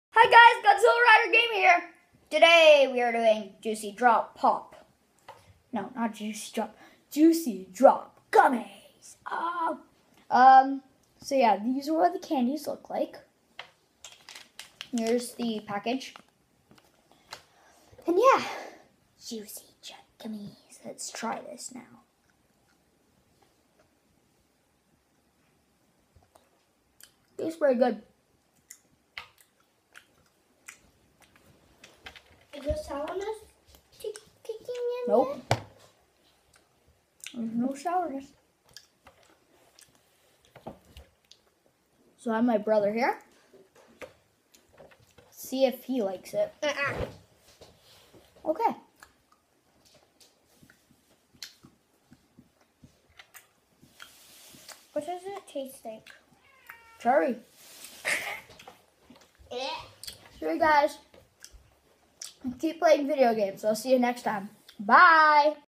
Trying JUICY DROP GUMMYS!!!!!!!!!
Hi guys I know I haven't posted in a couple of weeks now :( So here you go with me Trying JUICY DROP GUMMYS!